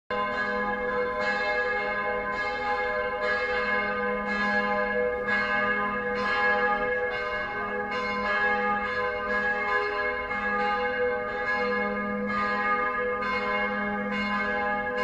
Bells ring at the Votive Church of Szeged
Votive Church of Szeged Bell Ringing at 6Pm on a Friday Night.